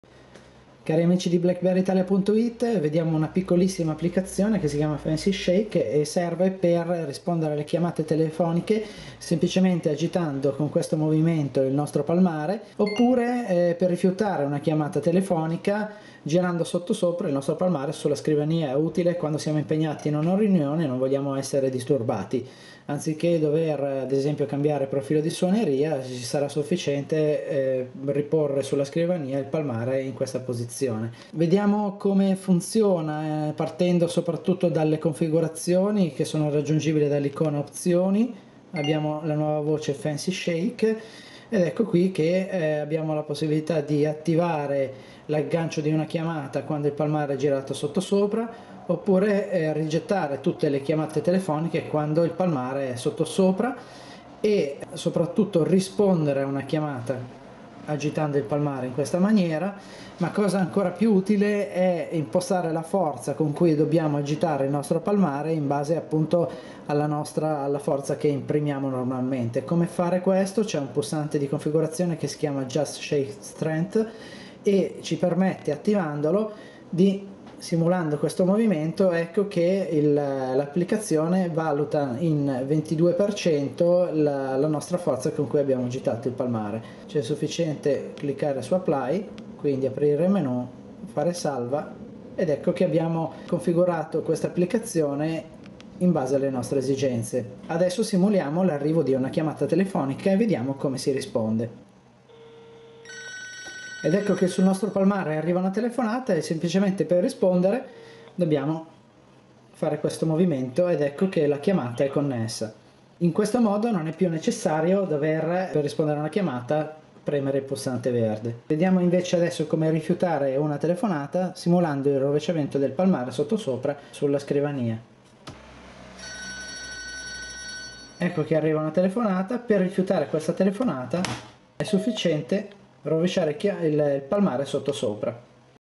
Fancy Shake per BlackBerry Storm, rispondere o rifiutare le telefonate con un semplice gesto
Fancy Shake e' un'applicazione per palmari BlackBerry RIM Storm 9500 e 9520 (Storm2) che permette di rispondere a una telefonata semplicemente "agitando" il nostro palmare oppure di rifiutarla capovolgendo il palmare sulla scrivania.